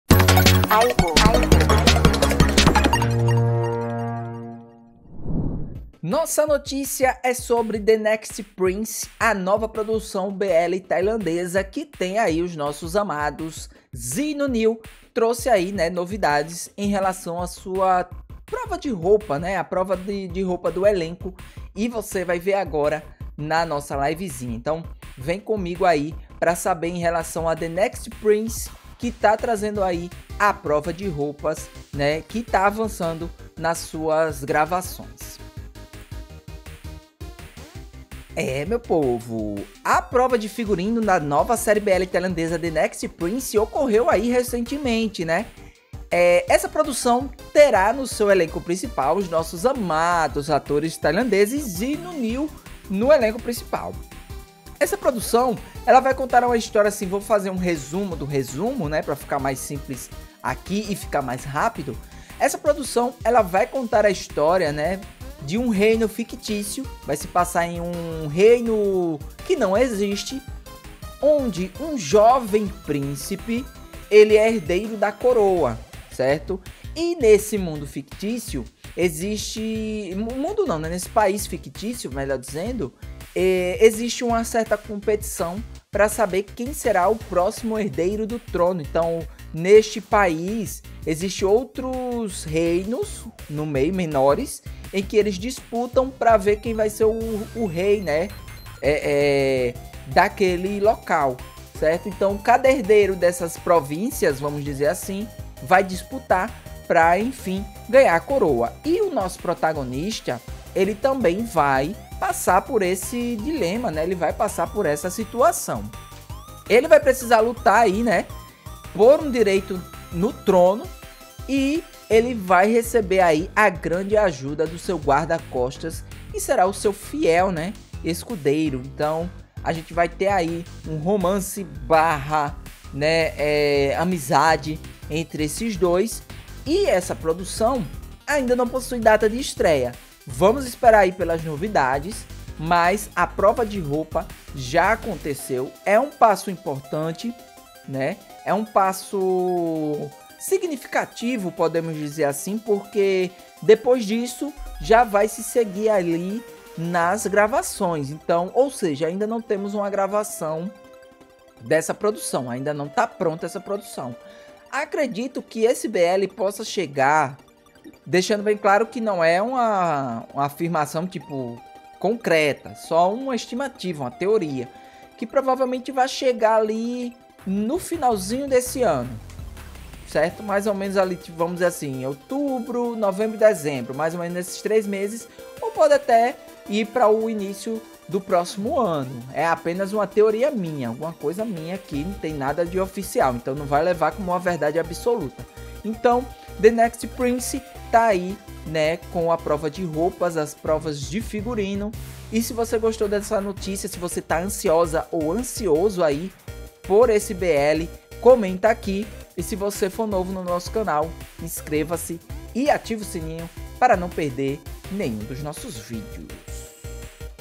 🚨Série BL "The Next Prince" realiza prova de figurino
A prova de figurino da nova série BL tailandesa "The Next Prince" ocorreu recentemente, com os queridos atores Zee e NuNew no elenco.
A história se passa em um reino fictício, onde um jovem príncipe, herdeiro da coroa, precisa lutar por seu direito ao trono com a ajuda de seu fiel guarda-costas.
A produção ainda não tem data de estreia.